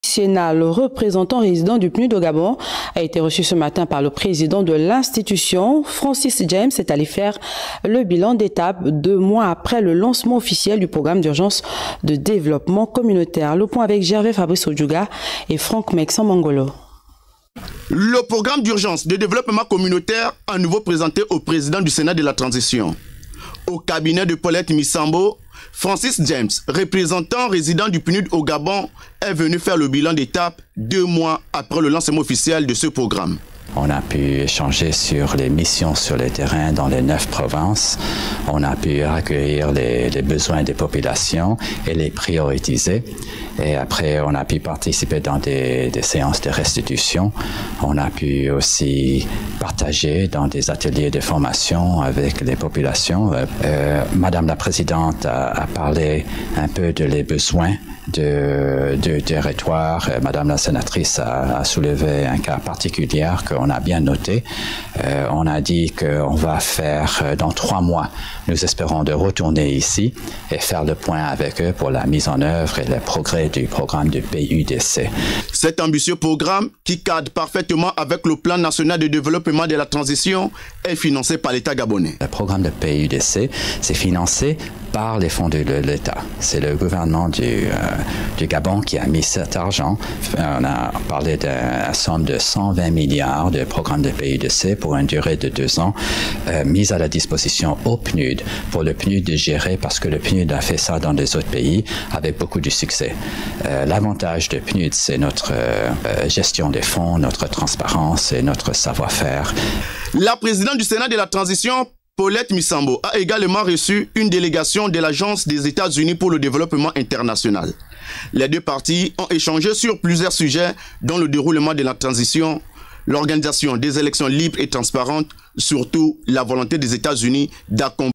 Paulette Missambo a reçu tour à tour Francis James le , une délégation des EtatsUnis
Paulette Missambo a reçu tour à tour Francis James le représentant résident du PNUD puis , une délégation des EtatsUnis pour le développement